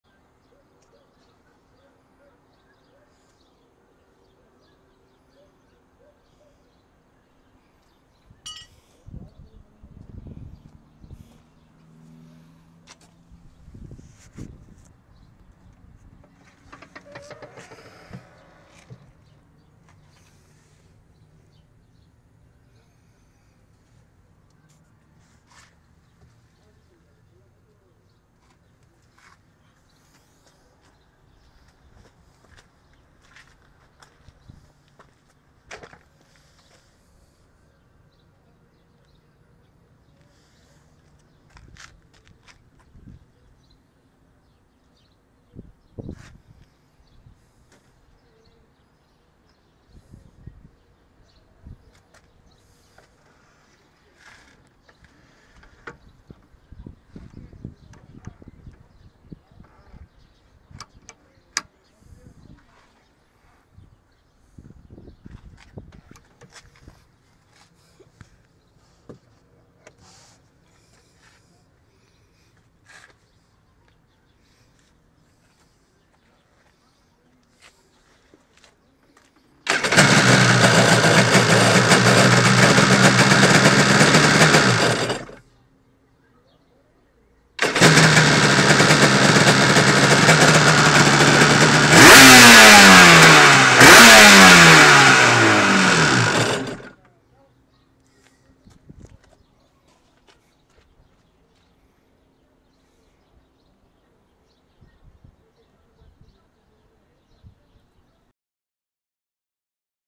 JS750SX 1995
MODIFICADO CON CABEZA,ESCAPE,CAMARA DE EXPANCION, MANIFUL DE ESCAPE,STATOR, BASE DE DIRECCION O HANDLE POLE AC